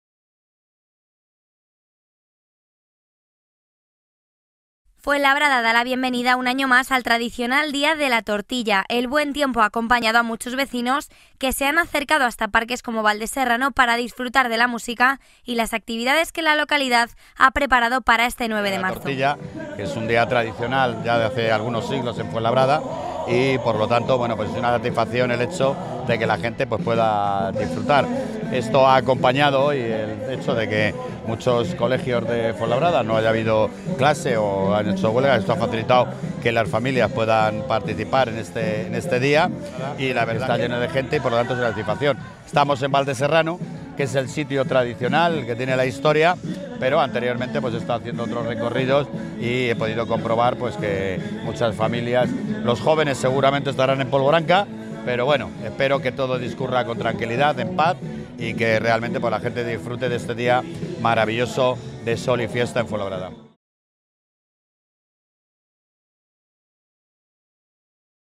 Fuenlabrada celebra el Día de la Tortilla
El alcalde Manuel Robles ha visitado Valdeserrano y otros parques de la ciudad en los que se celebra este tradicional día, y se congratula de que el buen tiempo haya animado a muchos vecinos a salir a celebrarlo.